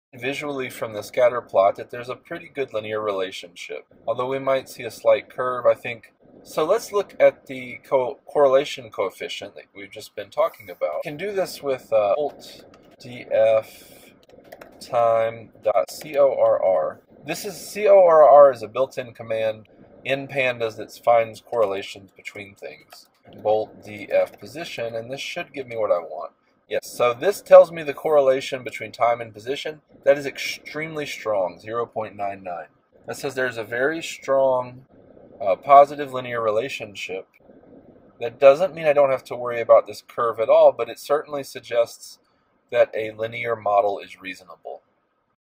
Machine Learning. Intro to Regression, part 5. Finding Correlation.
One tool for determining whetehr a linear model is a good idea for a particular data set is the correlation coefficient. When the correlation coefficient is  close to 1.0 or -1.0, we have a very strong linear relationship between two variables.